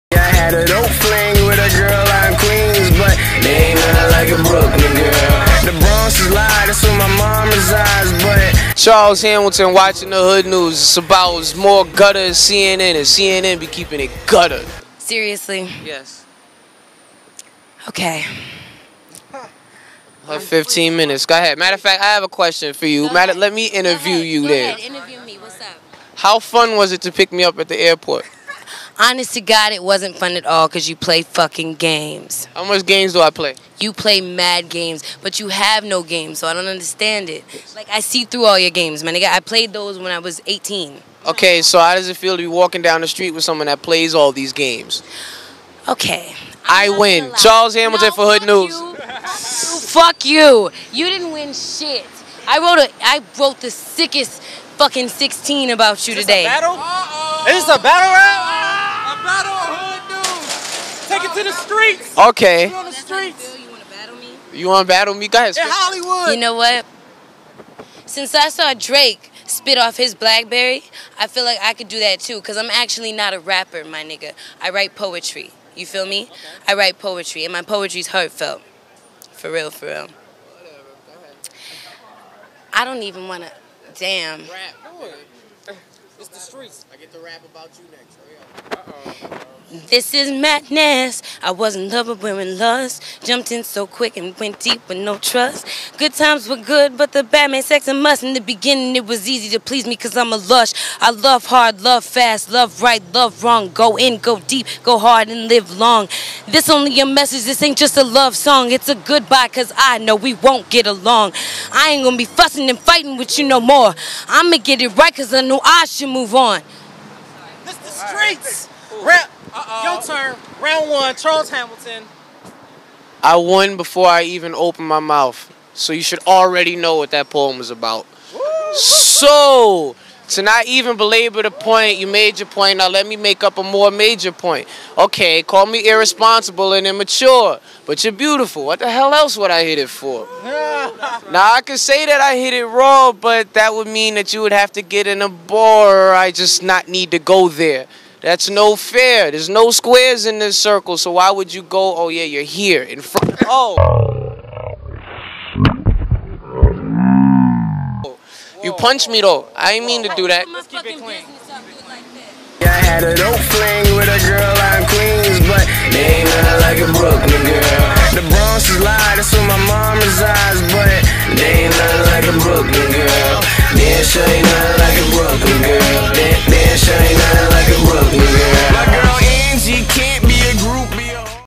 HoodNews: Charles Hamilton gets Punched in Battle Rap
Charles Hamilton gets Punched in Battle Rap